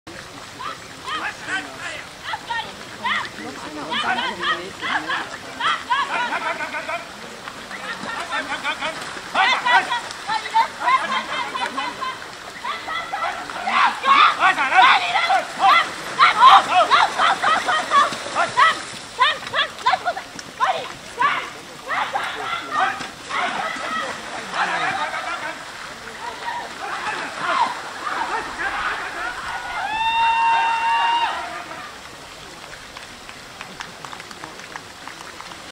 Fahrturnier Lähden (DJM) 3.8.2024
Ein kurzes Video einer Marathon-Fahr-Prüfung vom Fahrturnier in Lähden
Deutsche Jugendmeisterschaft
Bilder von verschiedenen Turniergelände bei Instagram:
Pferde.Flo